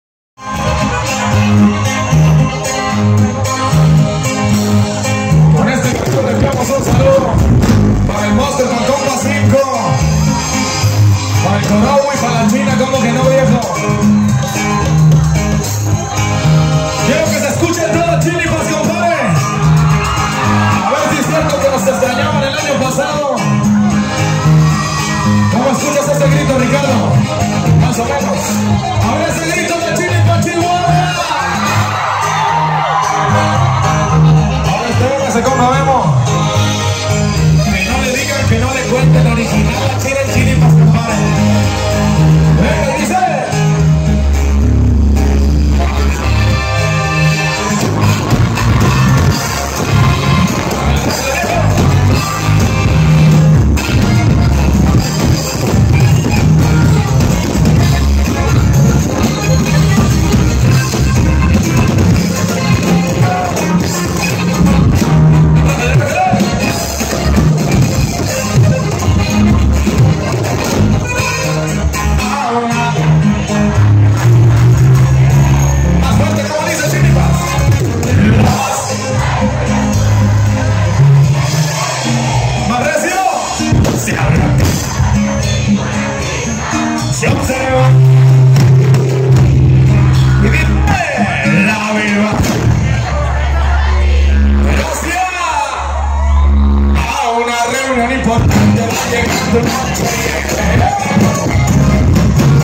Javier Rosas 2016 en Vivo en Chinipas Chihuahua
En vivo en Chinipas Chihuahua